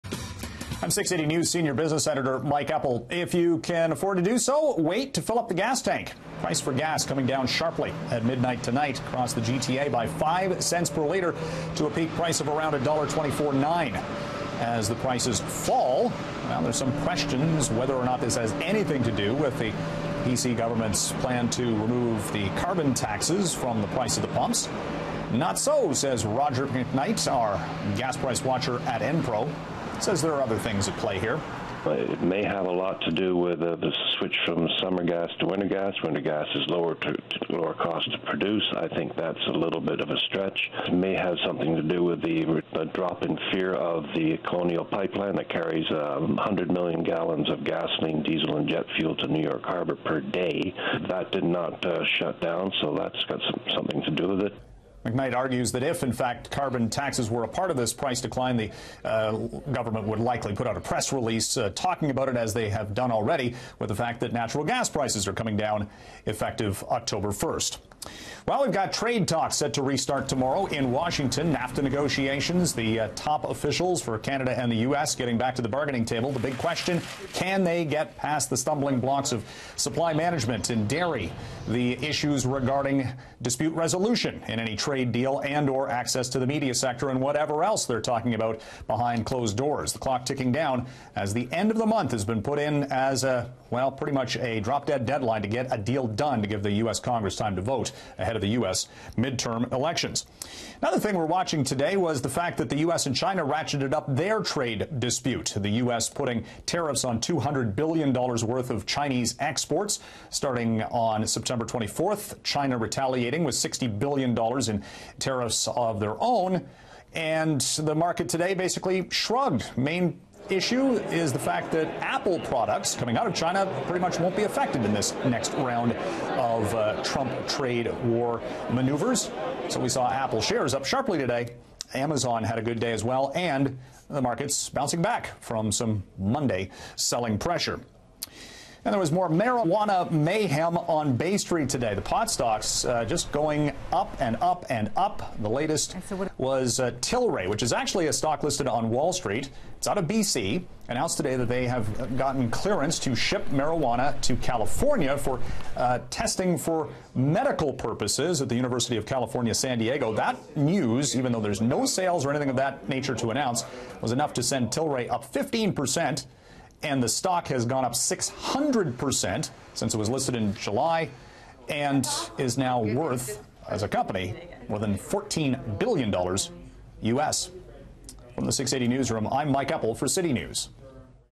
Business Report: Gas prices dropping 5 cents at midnight
The price of gas is expected to drop five cents at midnight. Meanwhile, NAFTA negotiations are set to resume in Washington, and the U.S. – China trade dispute intensifies. Mike Eppel has the details in today’s business report.